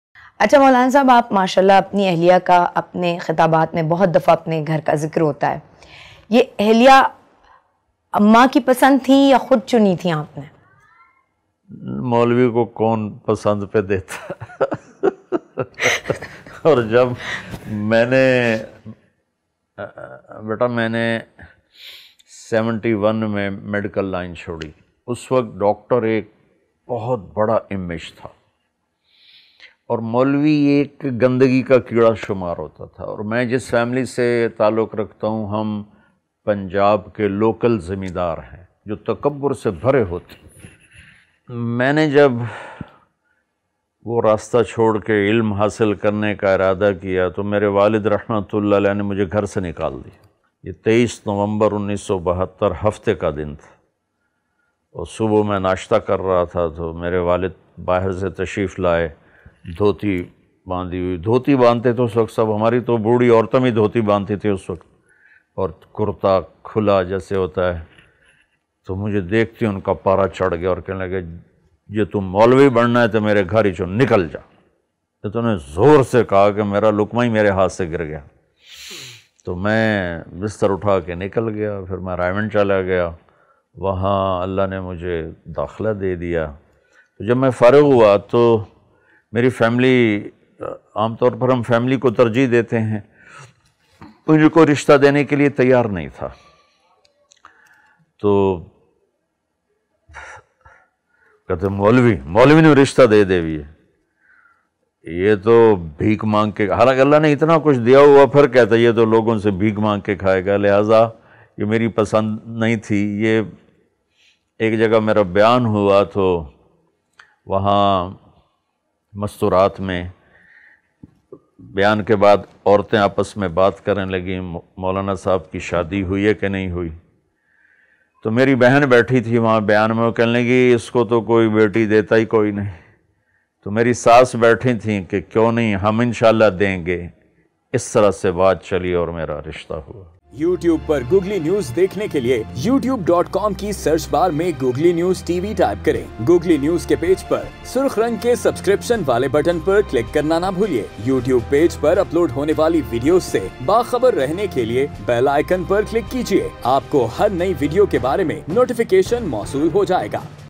Maulana Tariq Jameel Narrates The Story Of His Marriage | Googly News TV
Maulana Tariq Jameel Narrates The Story Of His Marriage